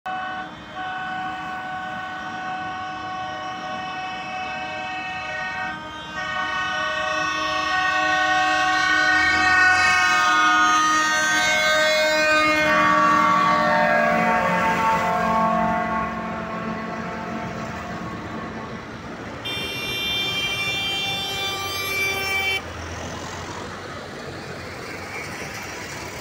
Πέρασαν τη γέφυρα της Χαλκίδας τα τρακτέρ από τη βόρεια Εύβοια
Ολοταχώς προς το σημείο συνάντησης με τους αγρότες που κατεβαίνουν από το Κάστρο Βοιωτίας, κατευθύνονται οι αγρότες της Εύβοιας.

Από τις 4 το πρωί έχουν ξεκινήσει και ήδη έχουν περάσει την γέφυρα της Χαλκίδας.